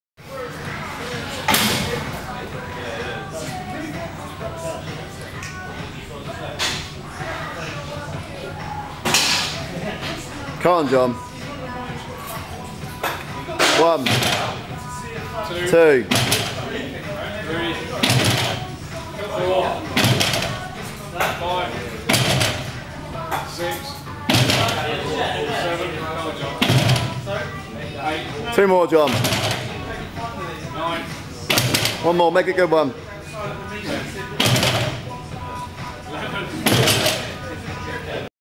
John Gallacher
John Gallacher doing 11 Reps in the 18 inch Deadlift with 160kgs at the Broadstone Sports Centre at Poole in Dorset at the Age of 80